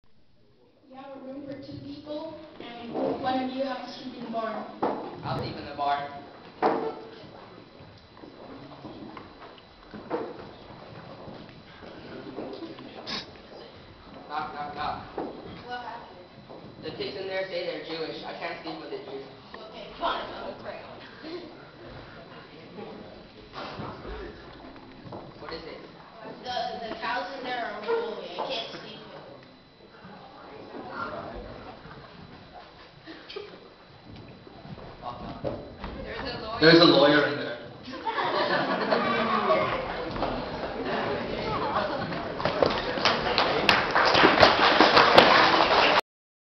Troop 452 New Year Party - Hawks Skit Part II
Lawyer, Nazi, and Hindu go to Farmer